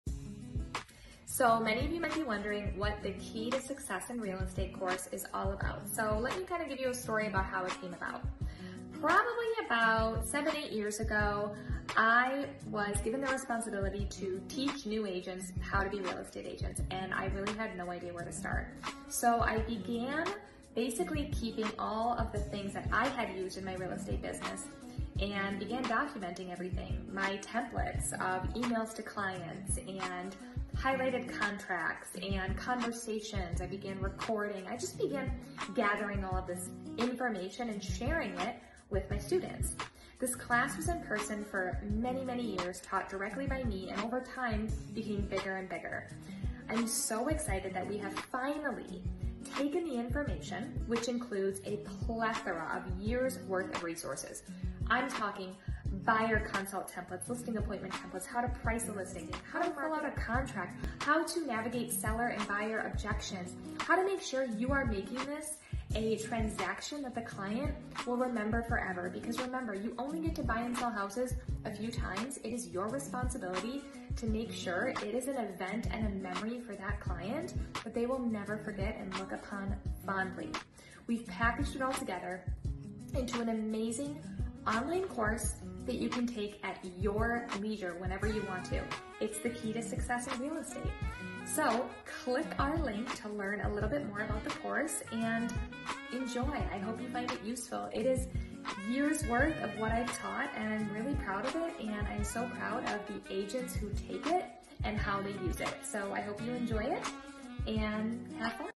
The Key To Real Estate Success
Unlock your potential with The Key to Real Estate Success, a comprehensive course designed to equip aspiring and seasoned agents with the essential knowledge and skills to thrive in today’s competitive market. Whether you’re new to the industry or looking to sharpen your expertise, this course covers everything from mastering client relationships and understanding contracts to leveraging modern marketing techniques and building long-term wealth through real estate.

Take the next step in your career and discover the tools, strategies, and insights needed to succeed in real estate!